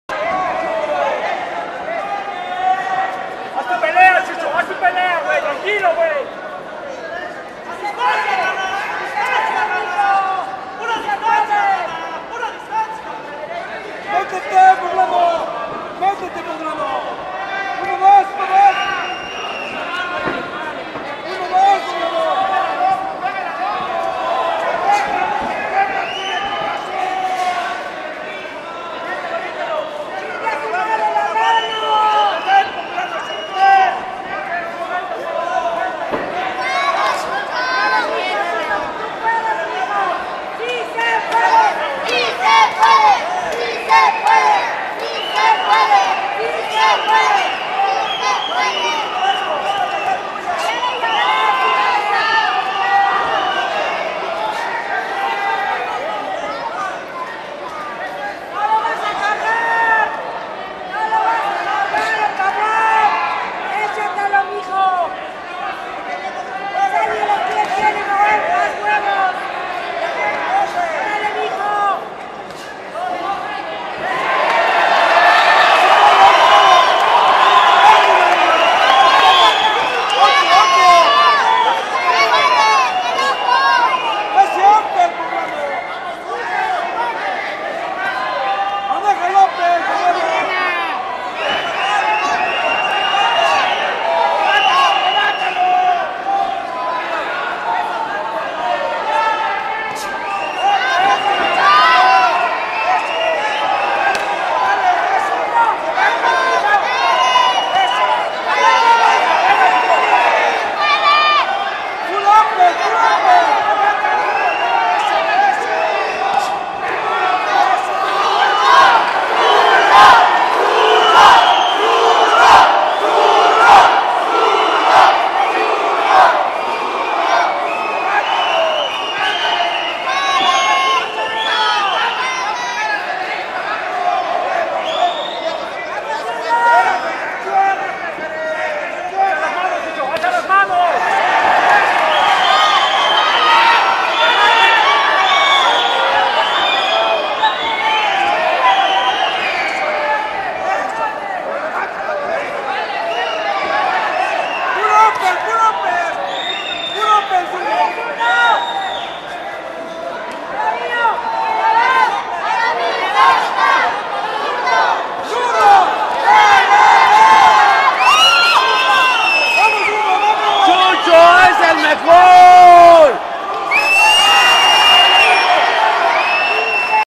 RANITA VS GALICIA 03
Excelente pelea entre Miguel Angel "Ranita" Gonzalez vs Jesús "Zurdo" Galicia en la Función "Con Paso Firme" en el Teatro del Pueblo en Cuautlancingo, Puebla